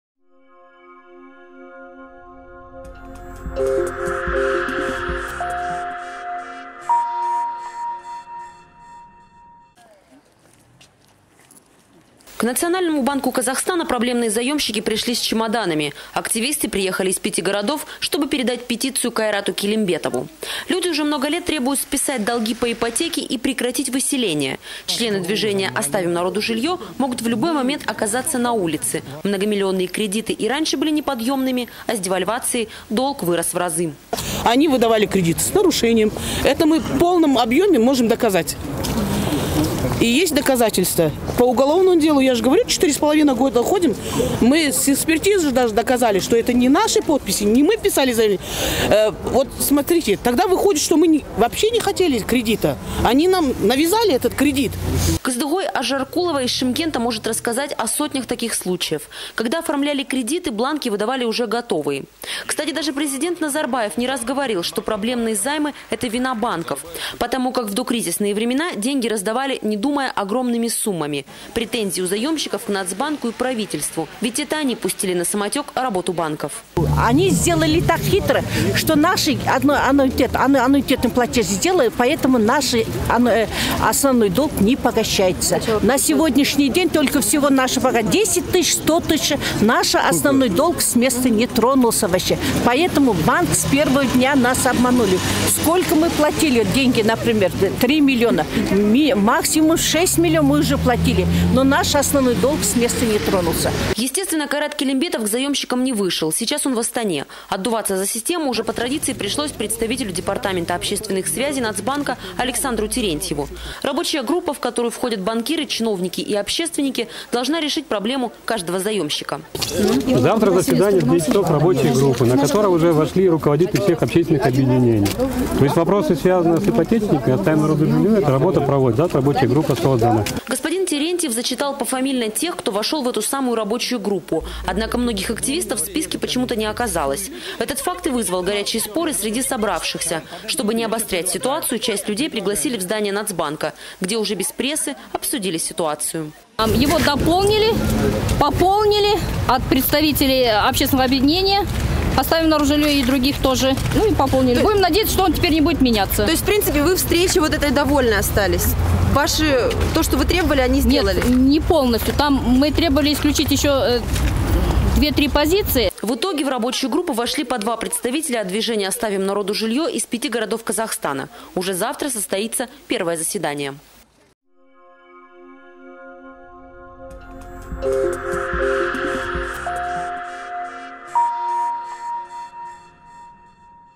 Ипотечники с чемоданным настроением / 1612
У Нацбанка собрались ипотечники. Активисты приехали из 5 городов, чтобы передать петицию Кайрату Келимбетову. Люди уже много лет требуют списать долги по ипотеке и прекратить выселения. Члены движения «Оставим народу жилье!» могут в любой момент оказаться на улице: многомиллионные кредиты и раньше были неподъемными, а с девальвацией долг вырос в разы.